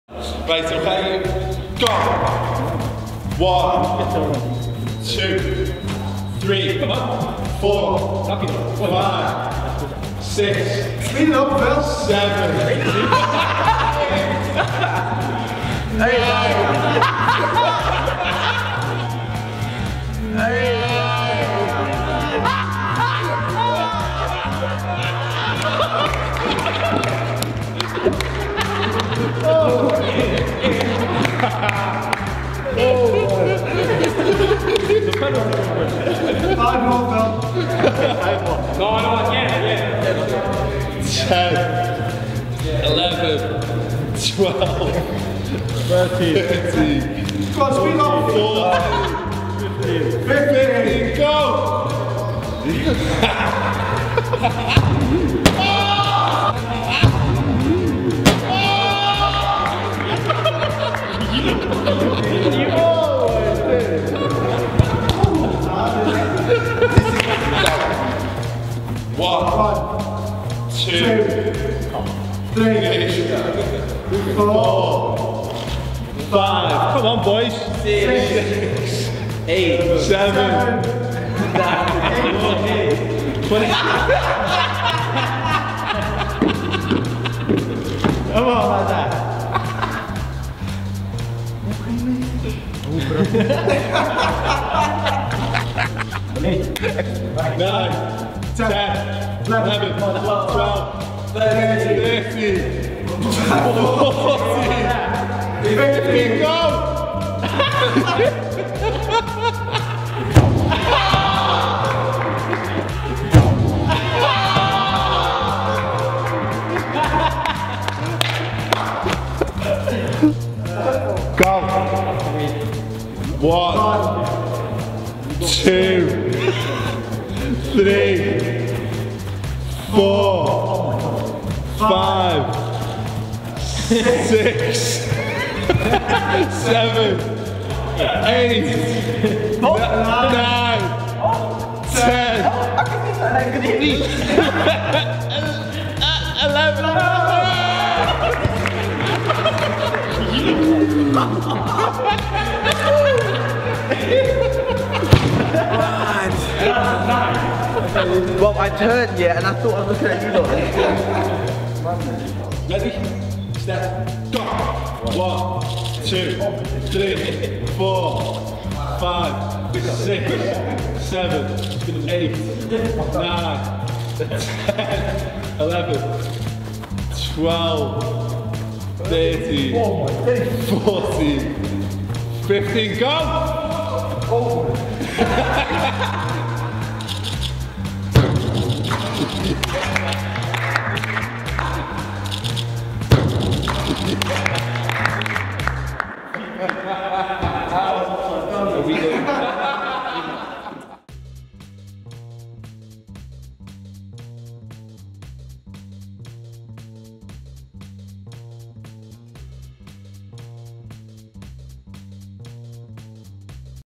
STERLING AND COUTINHO take Dizzy Pens for Dizzy Goals! | Redmen TV x Vauxhall Football
Phil Coutinho, Raheem Sterling, Lazar Markovic and Jordon Ibe face The Redmen TV Dizzy Pens Challenge! Watch their hilarious attempts to score past Redmen TV presenter Chris!

Raheem sterling falling over philippe coutinho lazar markovic Jordon ibe skills goals funny dizzy pens spinning